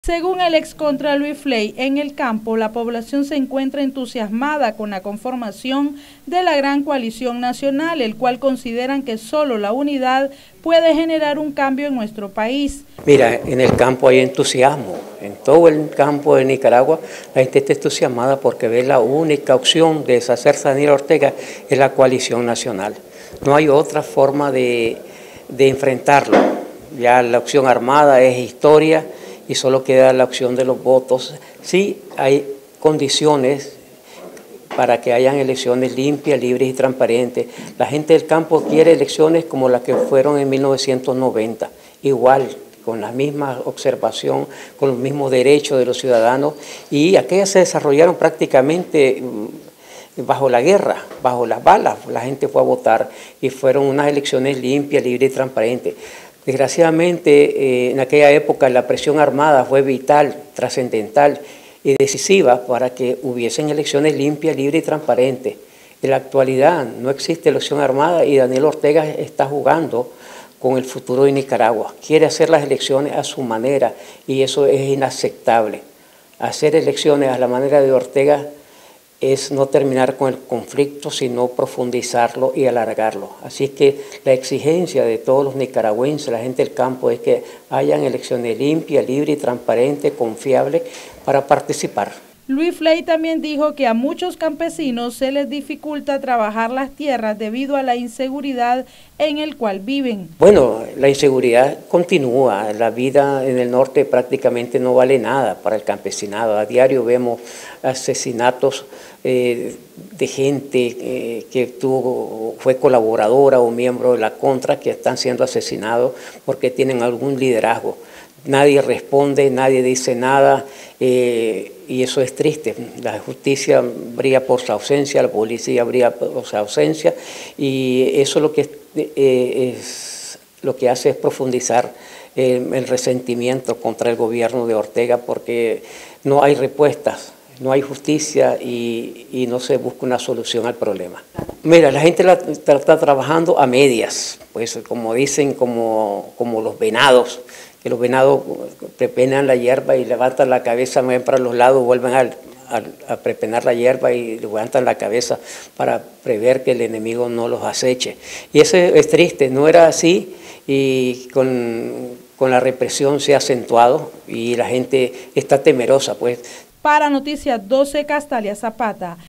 “Campesinos están satisfechos con la conformación de la Gran Coalición”
Los campesinos están satisfechos con la conformación de la Gran Coalición, dijo el ex contra Luis Fley.